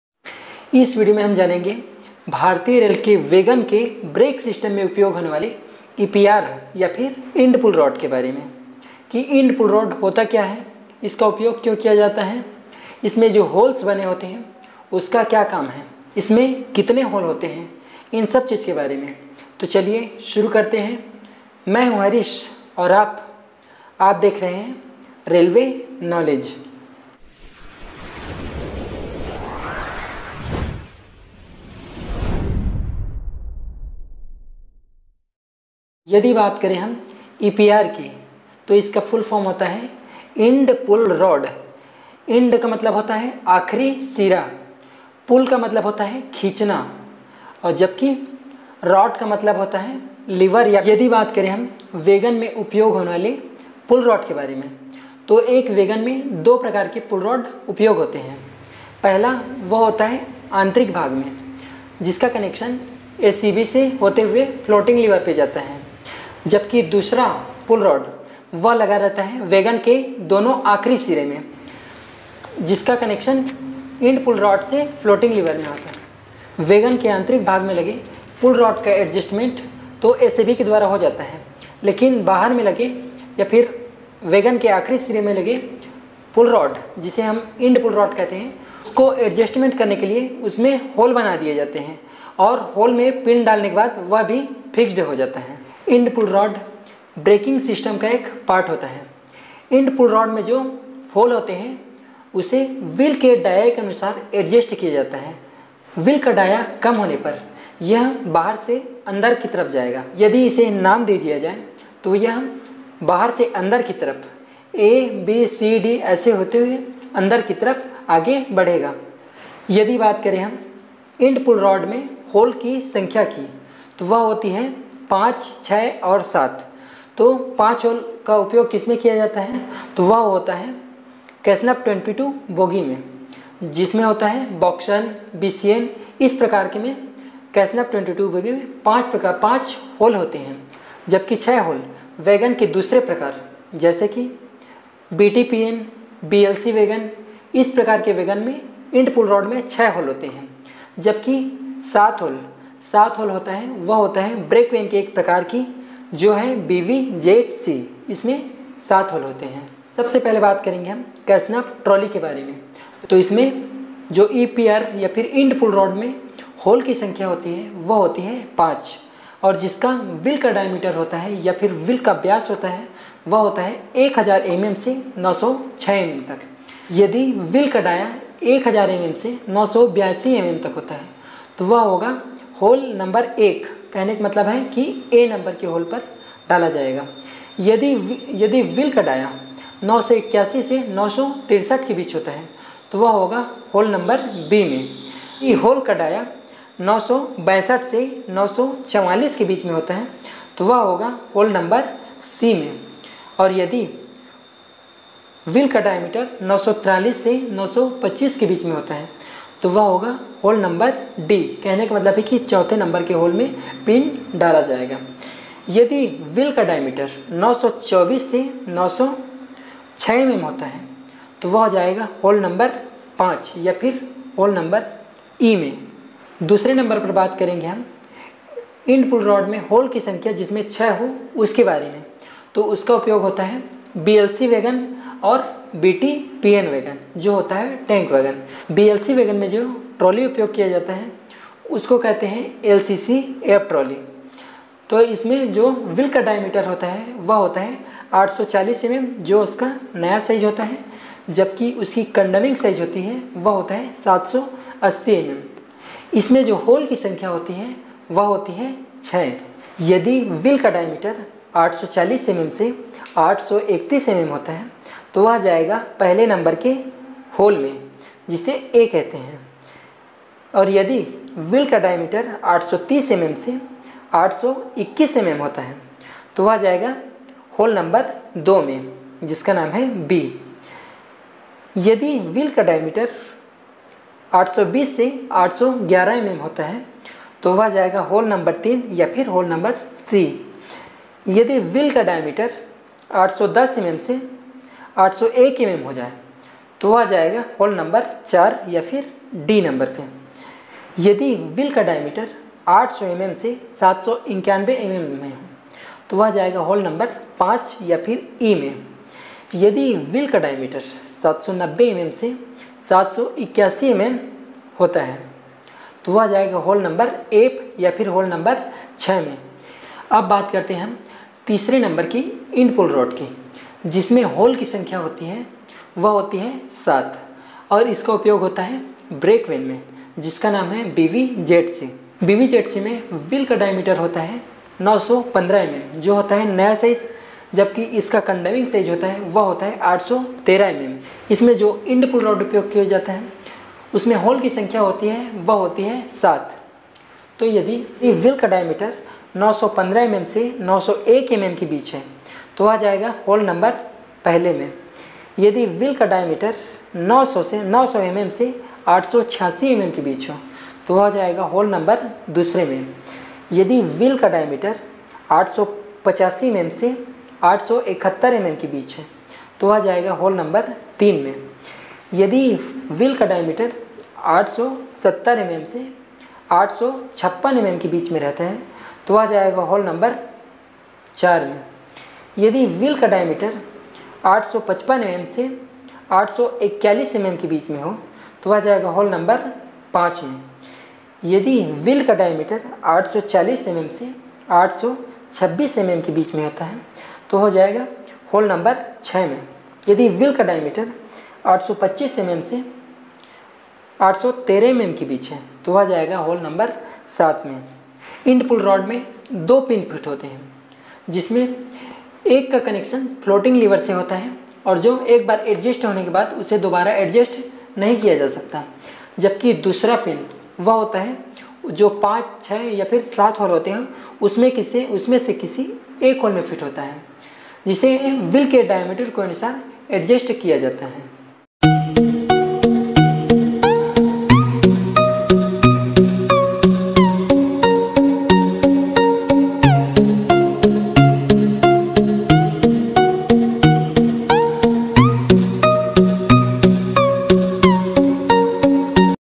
End pull rod ? | How to Adjust end pull rod hole ? | Types of End pull rod
इस वीडियो मे वैगन में उपयोग होने वाली end pull rod के बारे में बताया गया है

BVZI break van goods guard cabin ?

Thanks., friends for watch my channel.